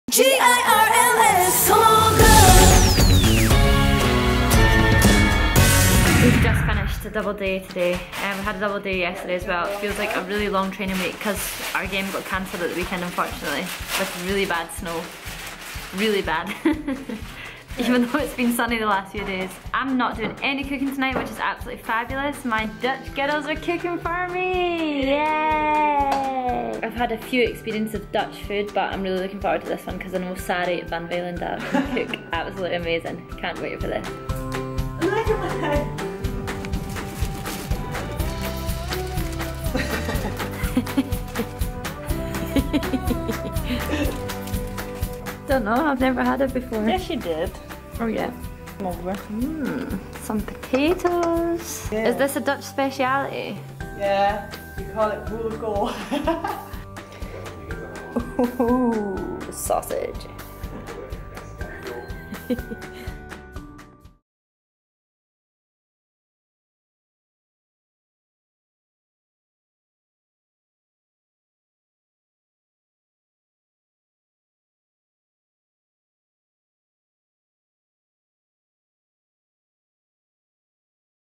Making Dutch food with Women's EURO Champions 🏆 | 👉▶️ Extra
Press Play Series One may be over BUT we have more!

Check out the best bits of series one every Thursday including training sessions, recipes and more 🙌😝

UEFA Women's Euro Champions Sari van Veenendaal and Vivianne Miedema join Scottish International, Lisa Evans for a Dutch cooking lesson!